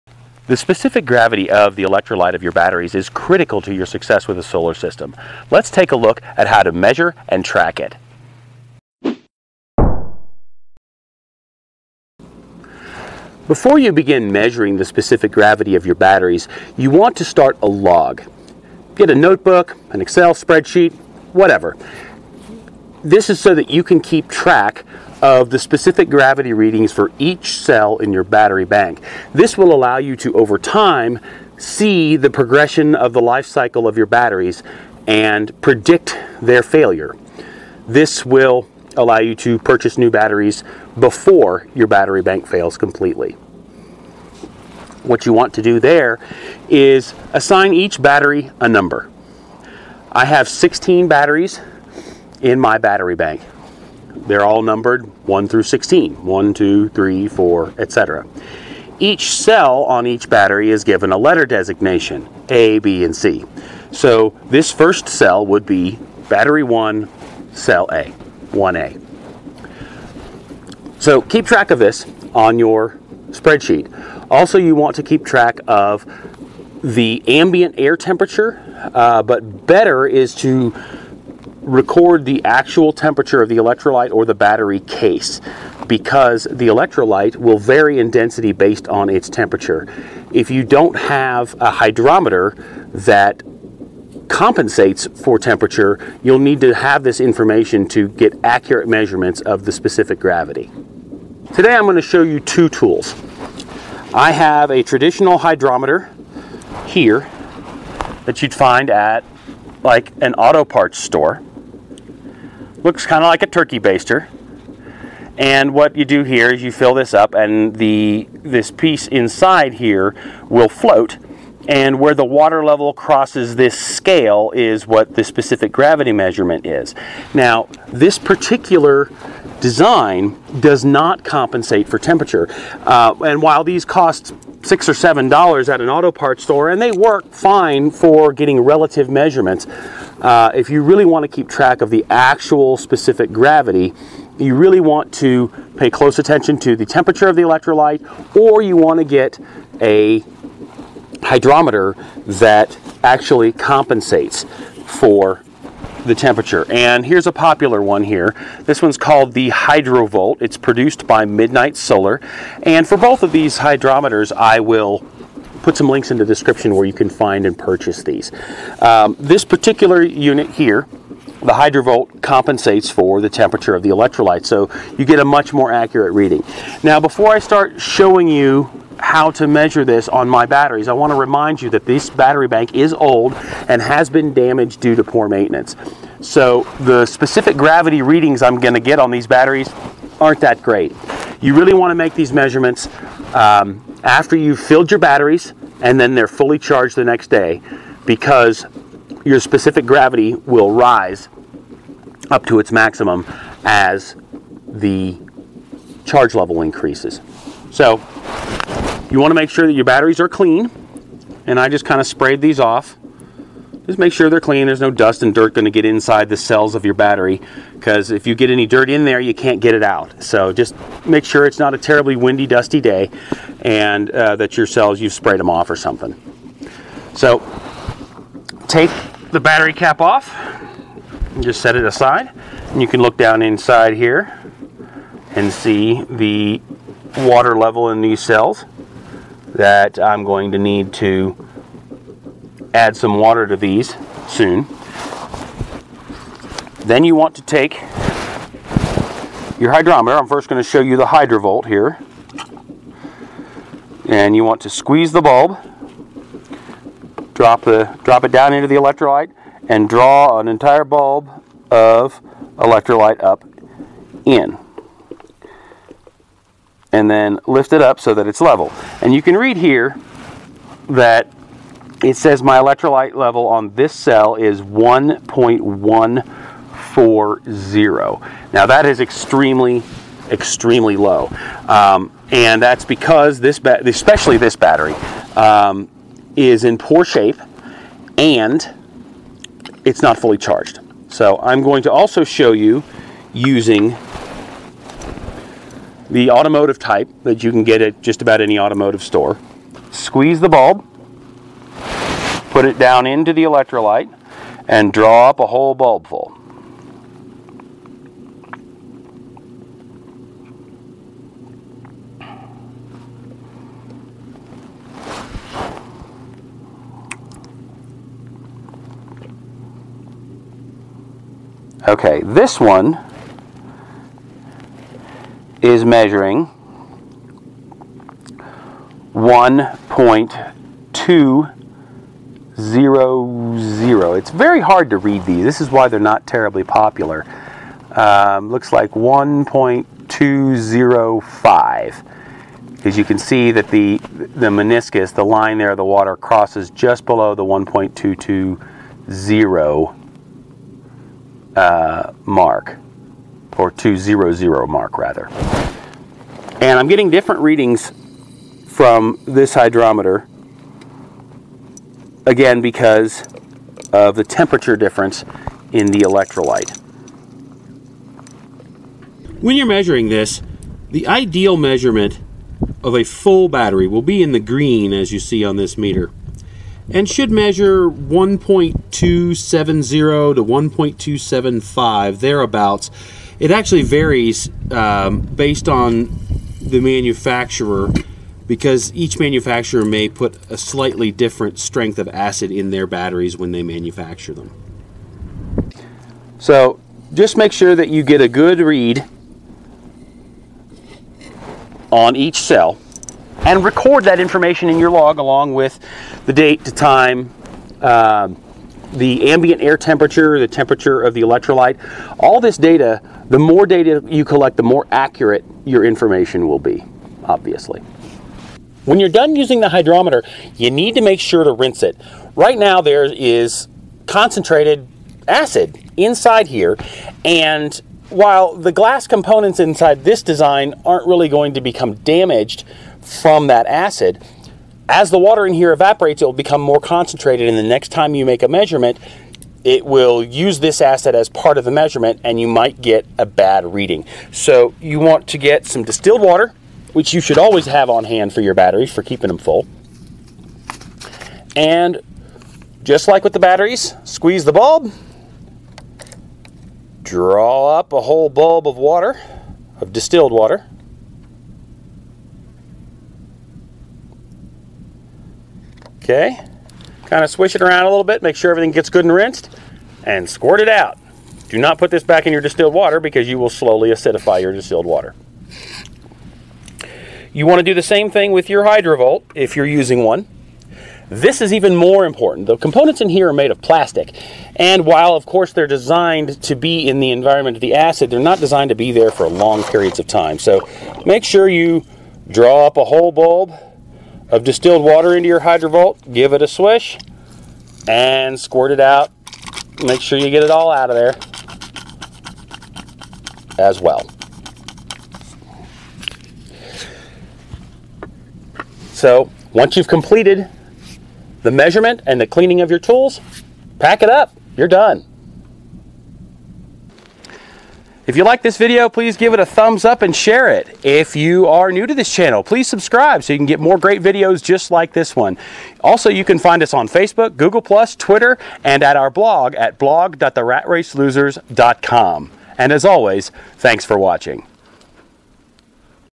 Measuring and Tracking Specific Gravity in Lead Acid Batteries
Learn how to measure and track the strength of the acid in your off-grid solar system's batteries.

Standard Hydrometer

Hydrovolt Temperature-Compensating Hydrometer

Leave us comments on what you'd like to see in future videos.

(RRLXXX)